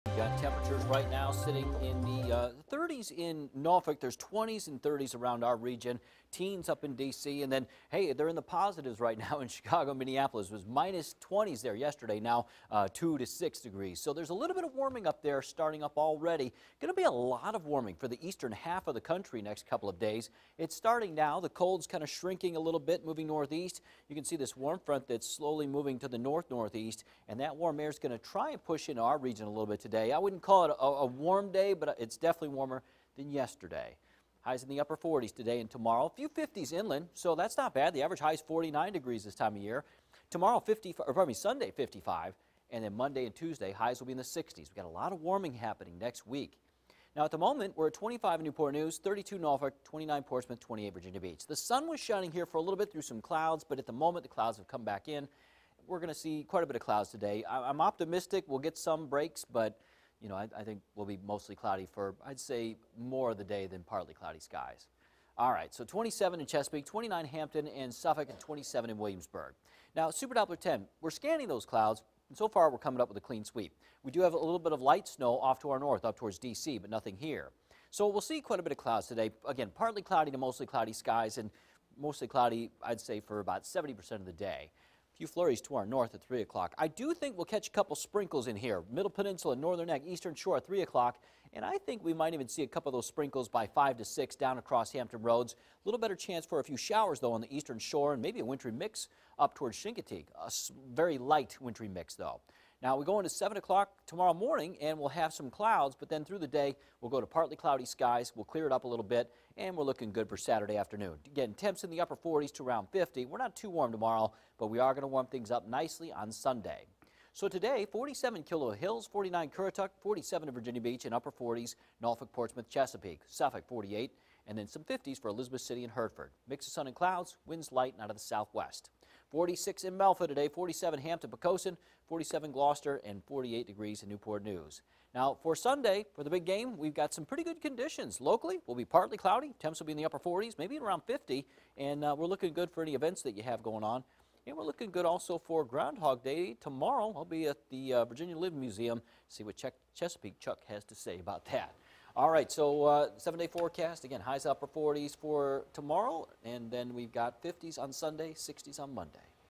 On the up and up
After the cold blast, temperatures will warm up nicely over the next few days over the eastern half of the country. Meteorlogist Jeremy Wheeler reports.